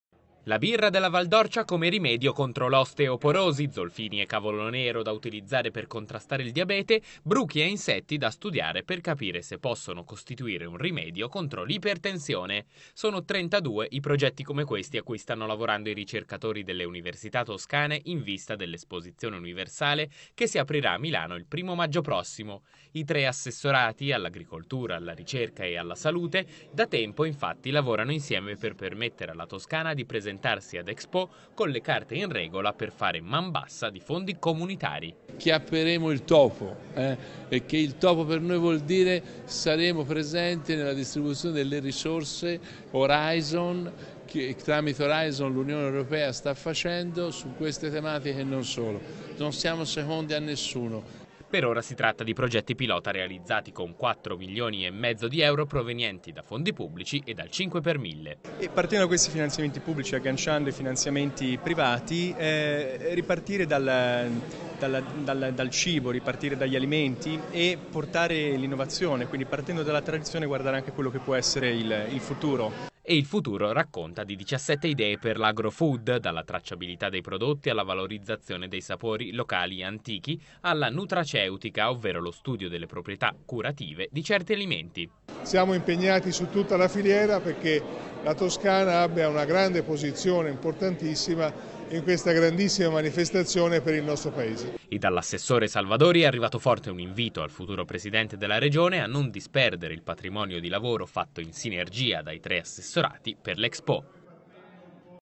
EXPO, LA RICERCA TOSCANA TRA CIBO E SALUTE
Dalla birra che cura l'osteoporosi al cavolo nero che combatte il diabete. Ecco i 32 progetti di ricerca che la Toscana vuole portare a Milano